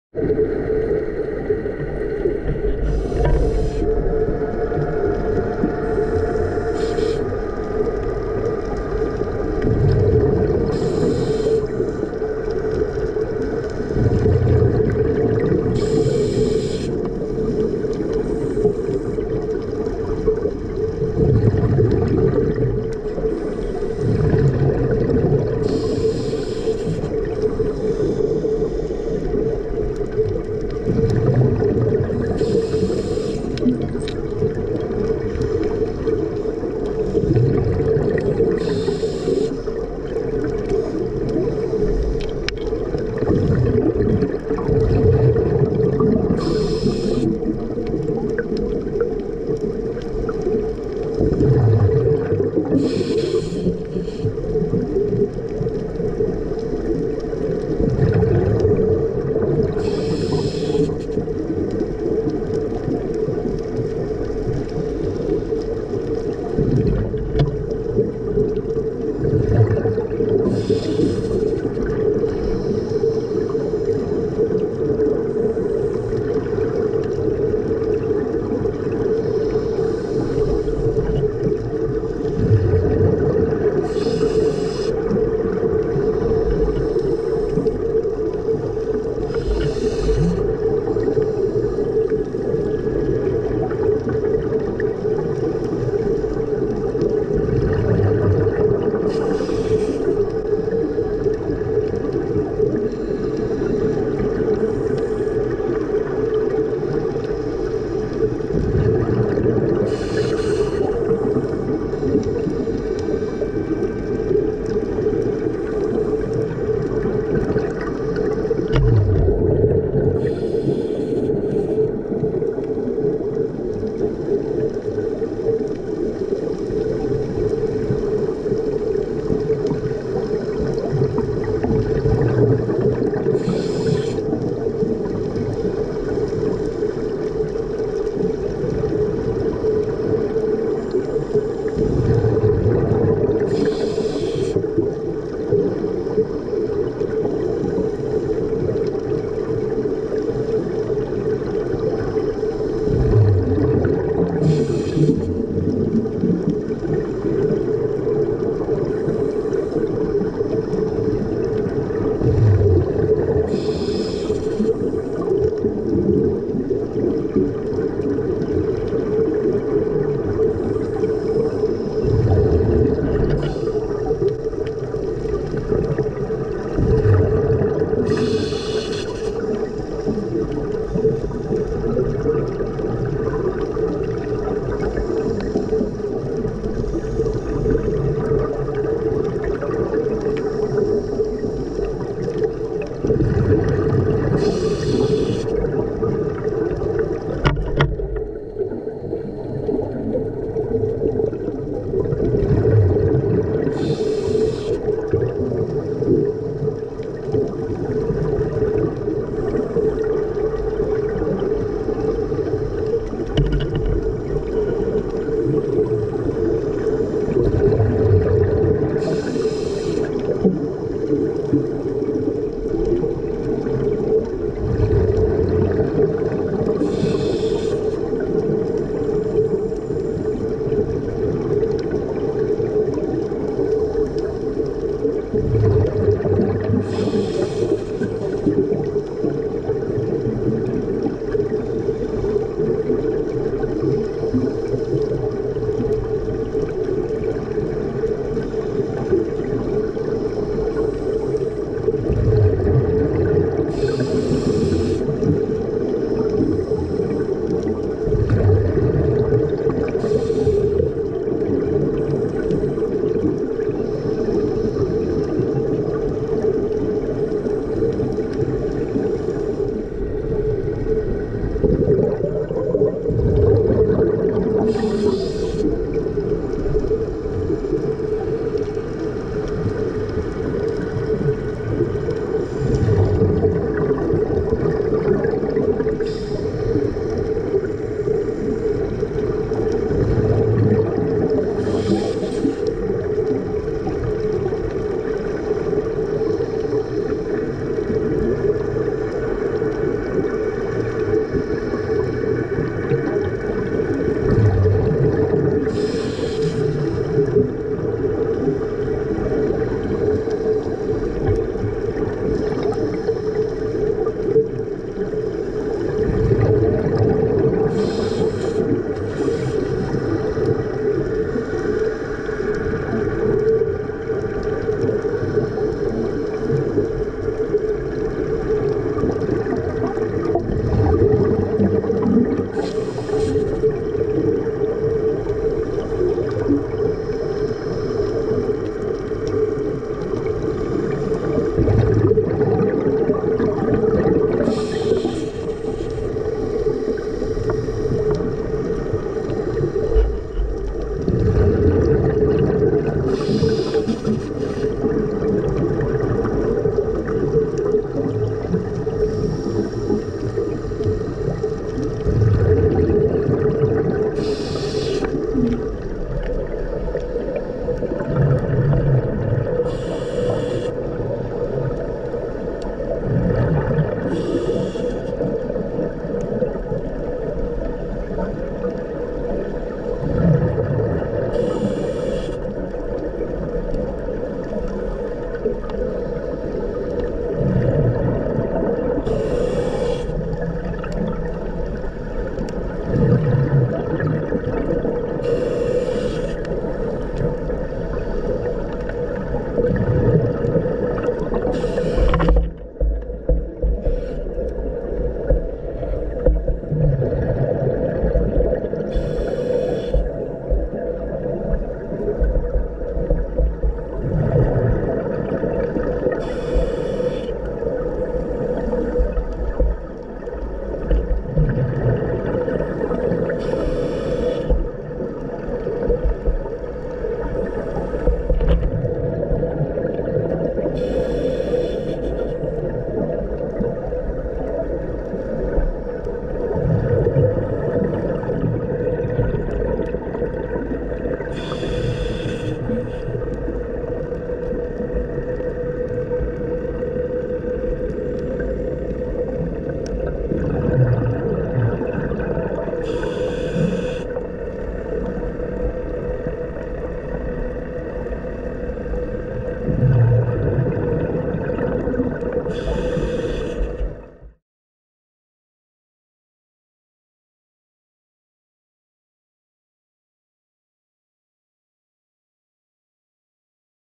Mantha Rays - Maldives Feb 2024
Images capturing the majestic beauty of Mantha Rays from the Maldives in February 2024.